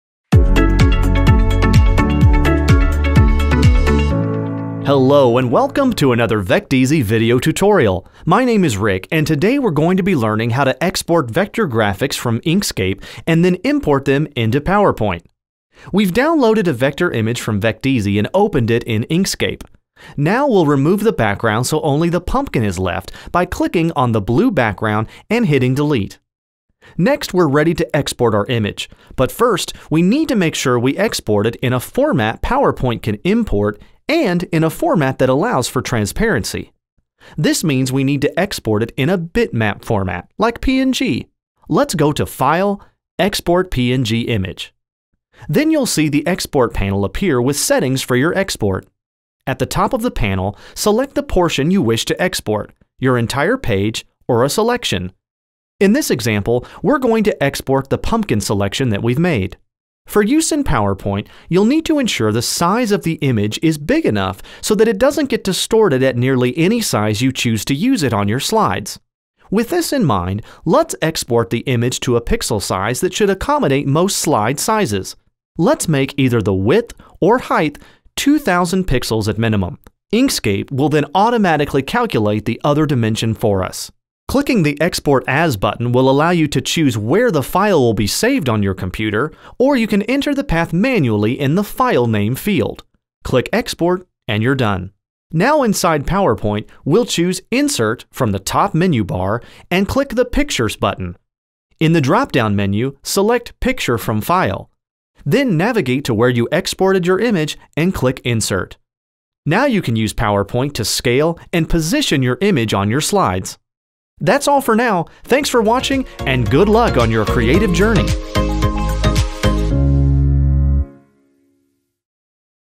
Exporting Images from Inkscape for PowerPoint
Learn how to easily save and export your designs from Inkscape in the PNG format for use in PowerPoint and similar applications.